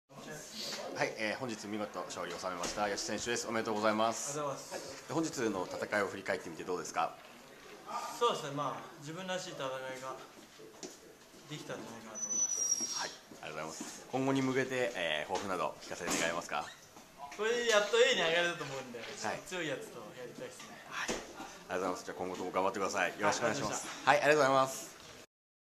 矢地祐介 試合後インタビュー 3.10 修斗後楽園
3月10日（土）東京・後楽園ホール大会で行われたSUSTAIN主催プロフェッショナル修斗公式戦で大澤茂樹を相手に見事勝利を修めた矢地祐介（KRAZY BEE）の試合後インタビュー動画です。是非、ご覧ください。